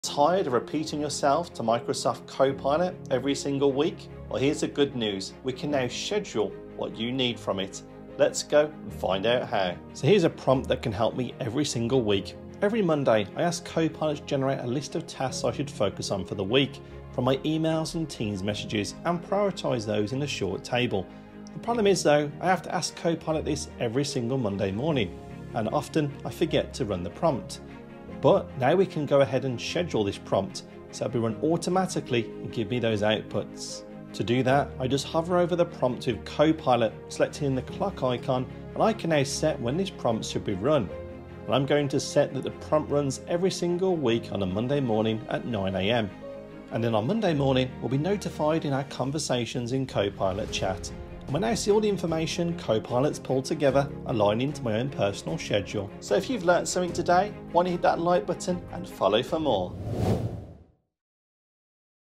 Schedule Prompts in Microsoft 365 Copilot and Save Time Every Week!
Stop repeating yourself to Copilot every week!

With Scheduled Prompts in Microsoft 365 Copilot, you can automate your most-used requests—saving time and boosting productivity.